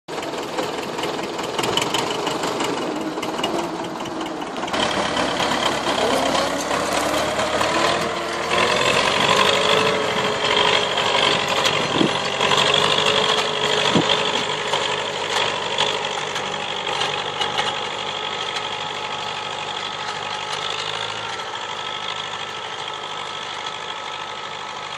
Curatare buruieni si bilonare lavanda BIO - Comana Constanta Romania Organic Lavender
Curatare buruieni si bilonare la lavanda BIO - Comana, Constanta, Romania